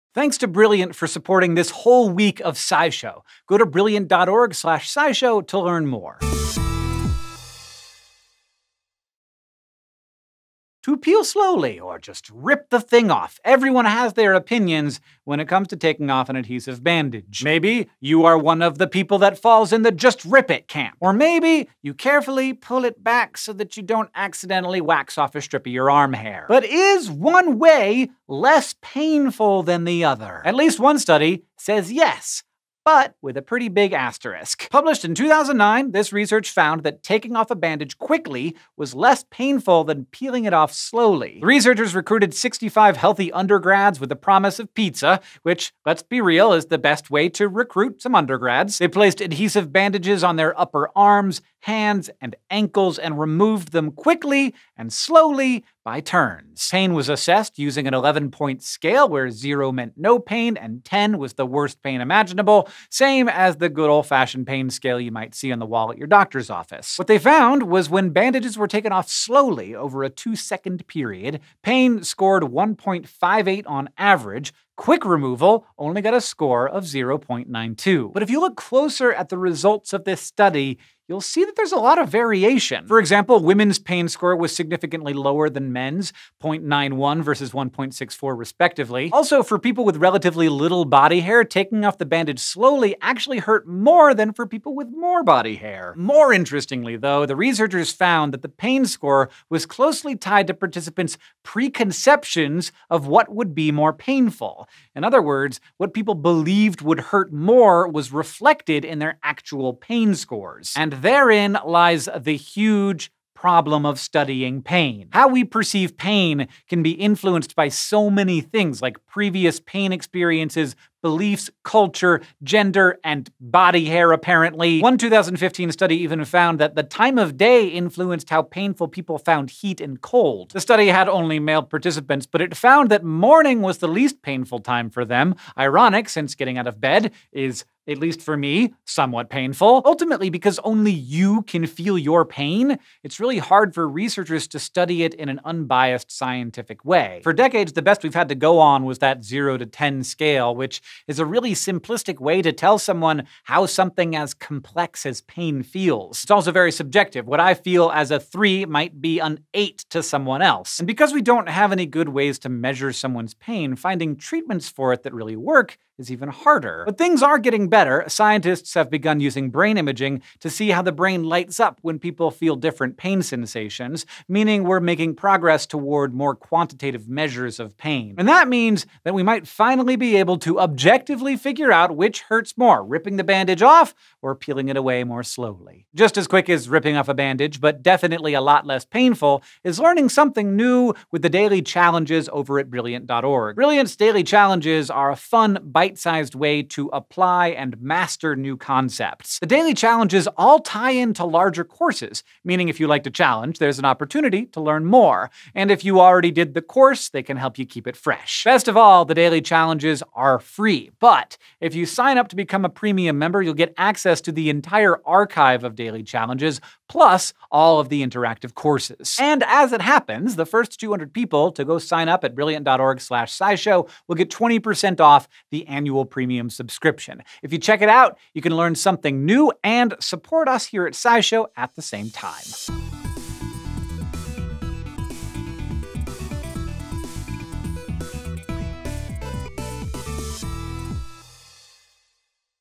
Does Ripping Off a Bandage Hurt Less?
To peel slowly or just rip it off, what is the best way to take off an adhesive bandage? One study found some interesting results.

Images